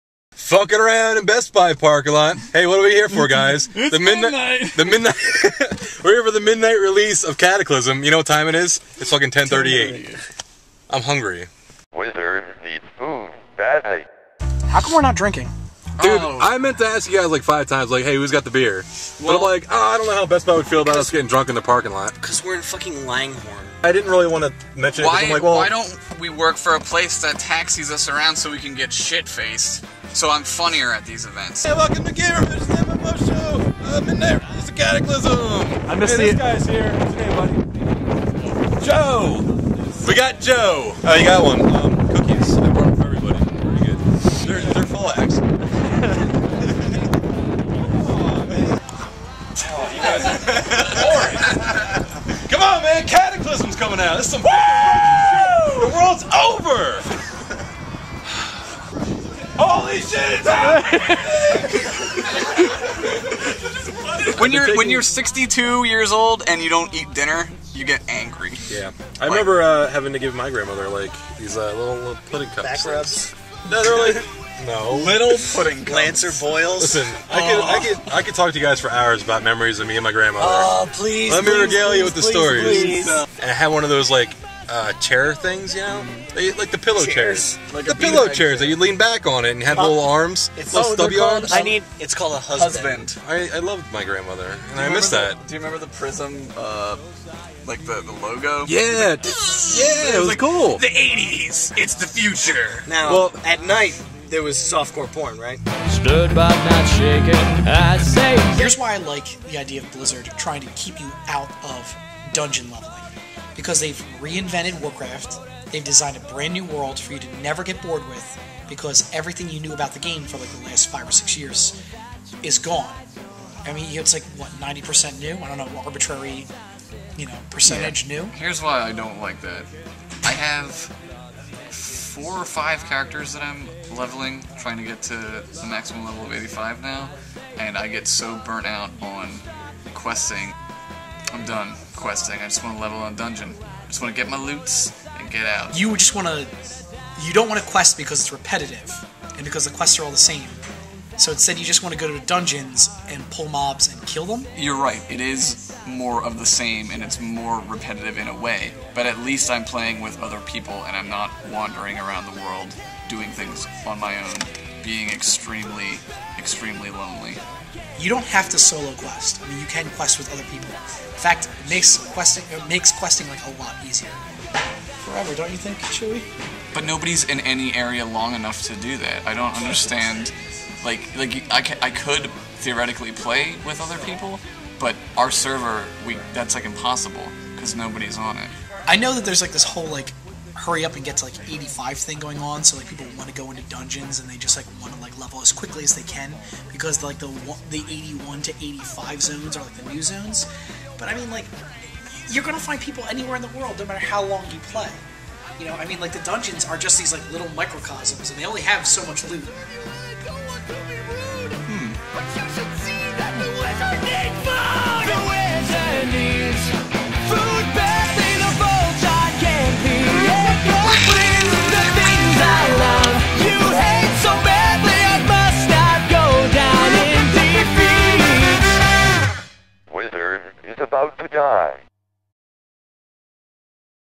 MMO Show Behind the Scenes Cataclysm Launch
As if you hadn't had enough of us already, The MMO Show crew is back on a Wednesday to bring you some bonus coverage from the Cataclysm launch plus a bonus discussion from yesterday's show, but on video!  So get your ocular cavities ready for the most handsome (fugly) podcast team on the planet.  It's like listening to the show...but with your eyes!

Starring:  Timmy Frisch, Josh Henderson, Mike Sadorf, and Frank Snyder

Edited by Mike Sadorf and Josh Henderson

Graphics by Paul Ritchey